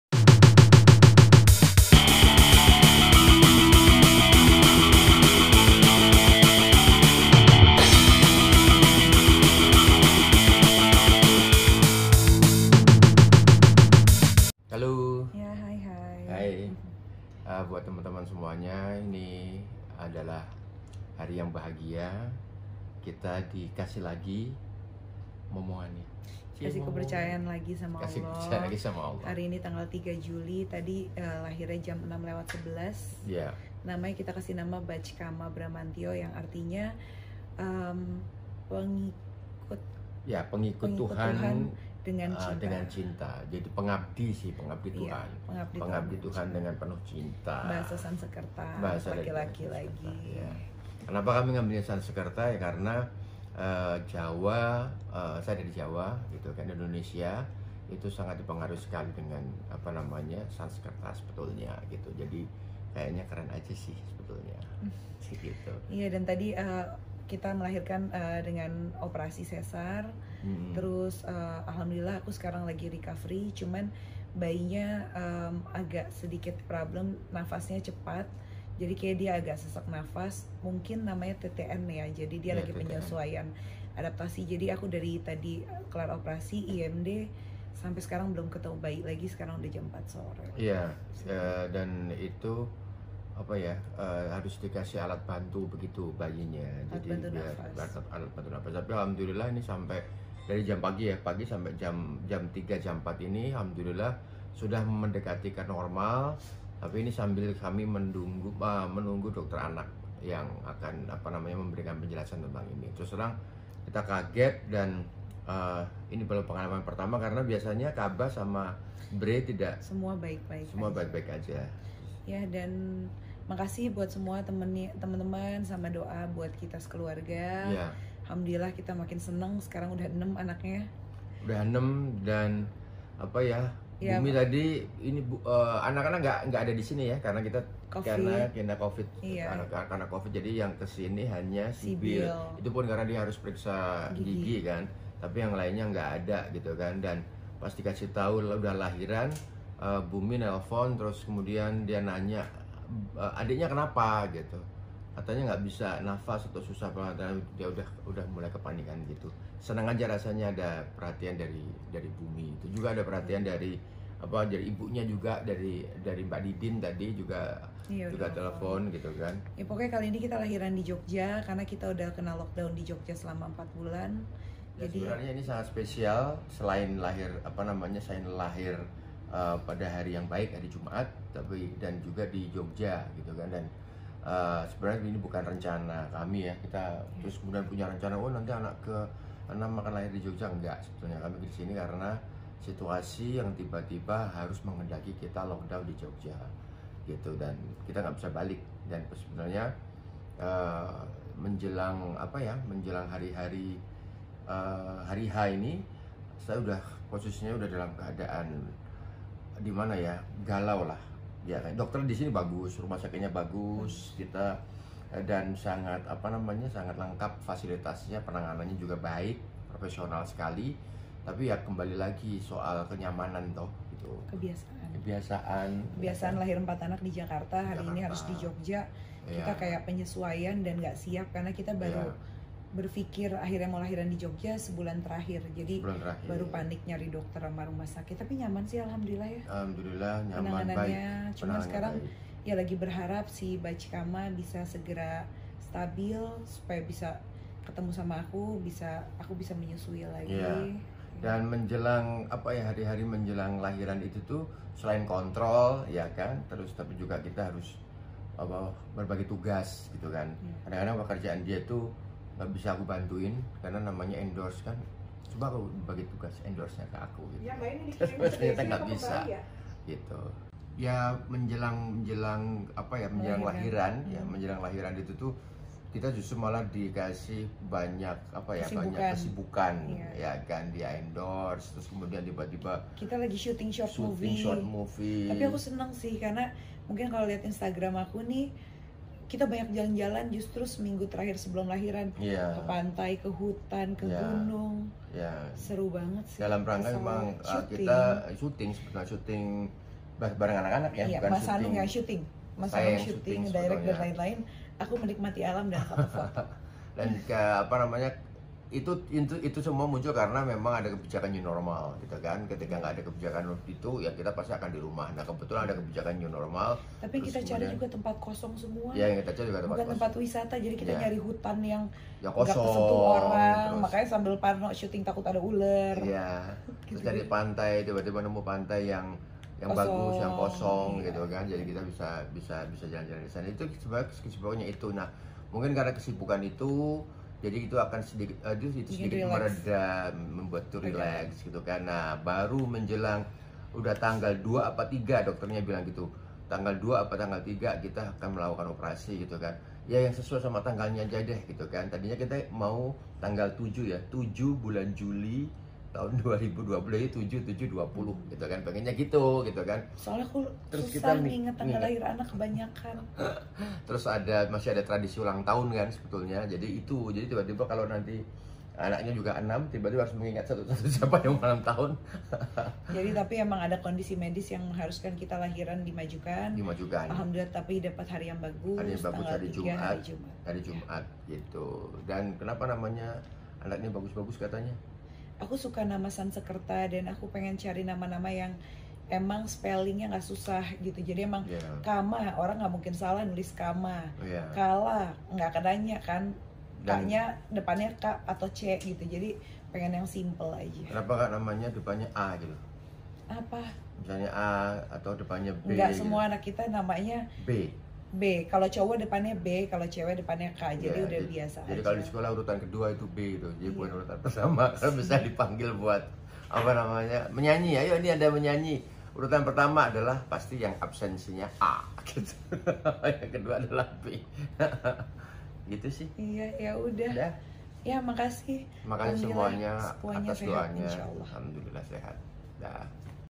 SELAMAT. Zaskia Mecca Lahirkan Anak Kelima di Jogjakarta
Kabar bahagia datang dari pasangan selebriti Zaskia Adya Mecca dan Hanung Bramantyo. Pada Jumat (3/7/2020) pasangan ini dikaruniai anak kelima yang berjenis kelamin laki-laki. Anak tersebut diberi nama Bhaj Kama Bramantyo